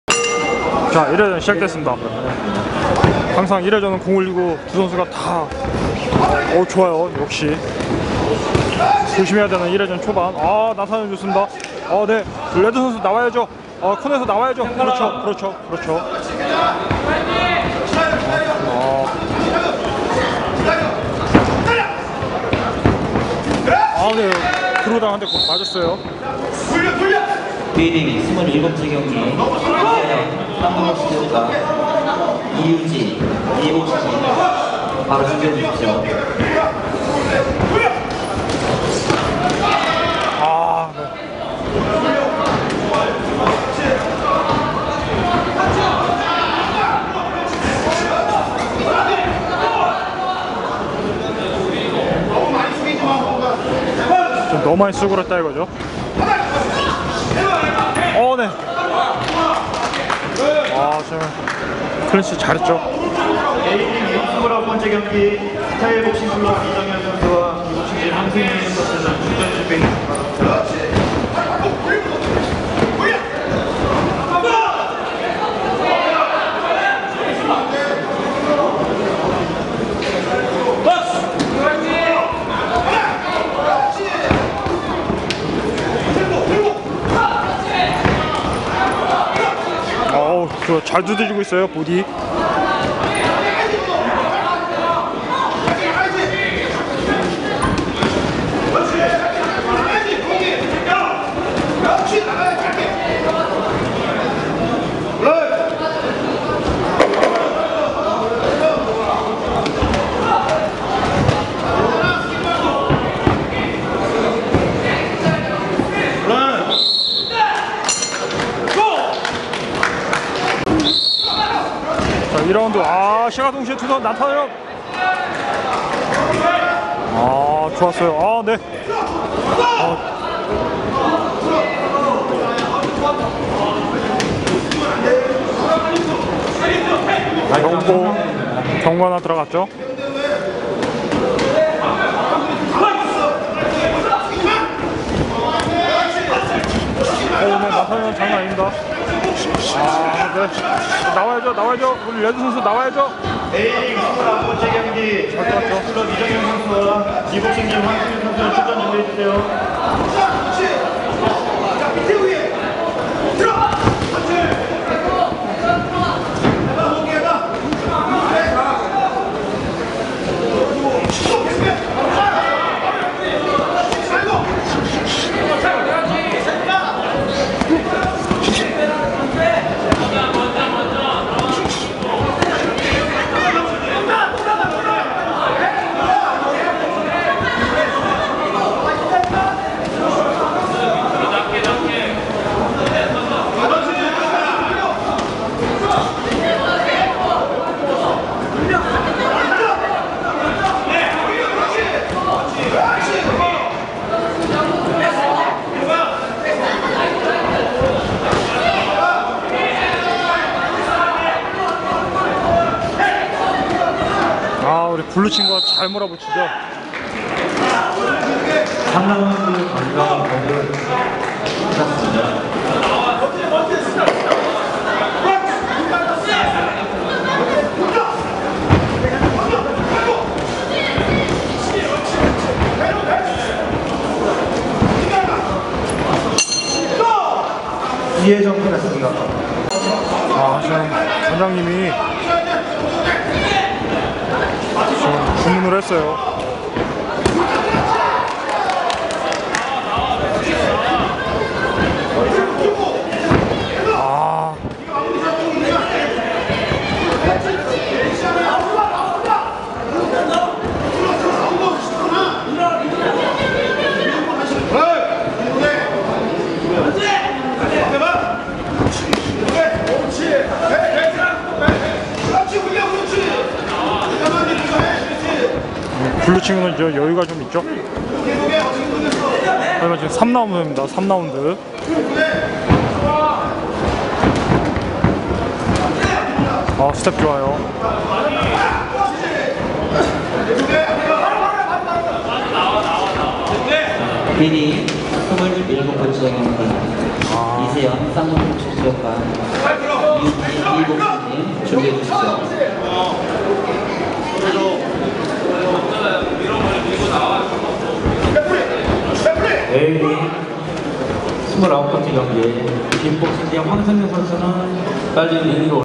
동대문구 생활체육대회 난타전!!! 노가드에 빠른 스피드!!! 블루 승 boxing sparring
동대문구 생활체육대회 난타전!!! 노가드에 빠른 스피드!!! 블루 승